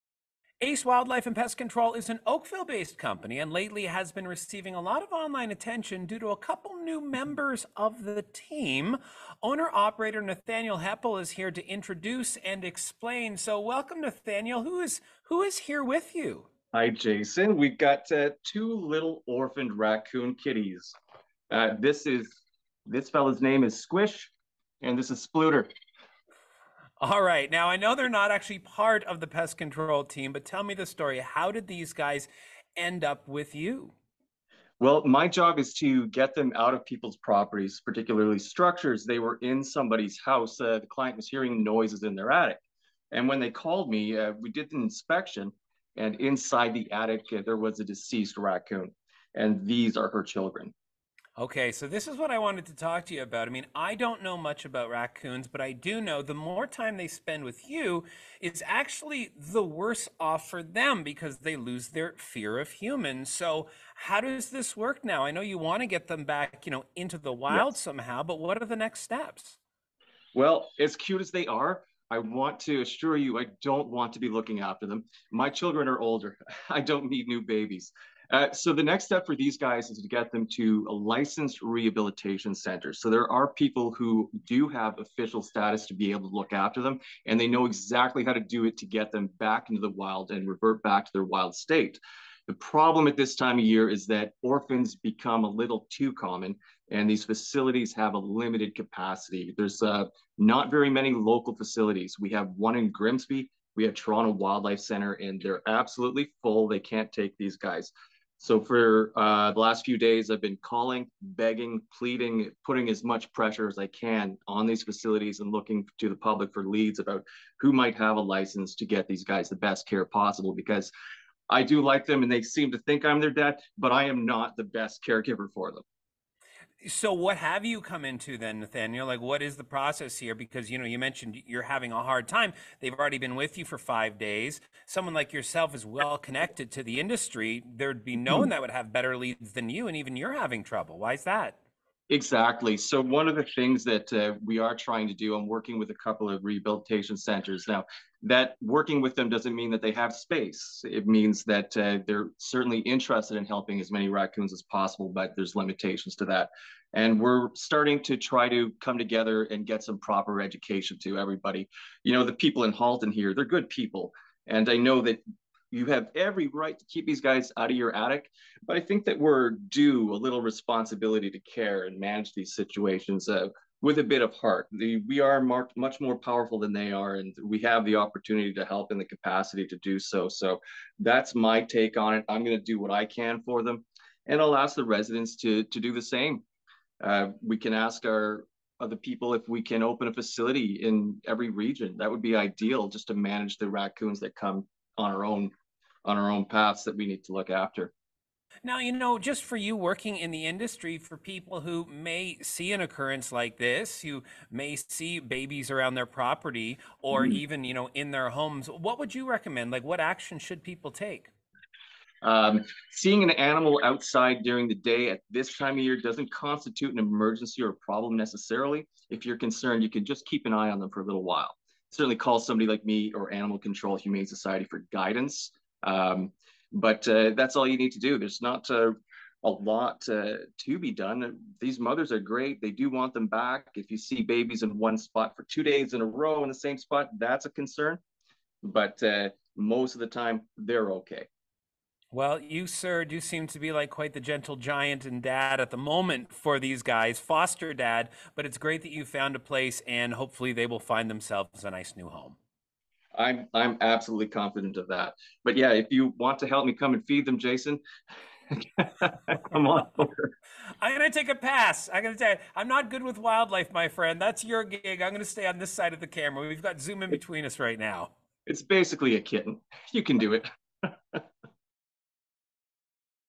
Orphan Raccoons in Oakville
Oakville resident Nathaniel Heppell of Ace Wildlife & Pest Control introduces us to two orphaned racoons and chats about his struggle to find them a proper new home.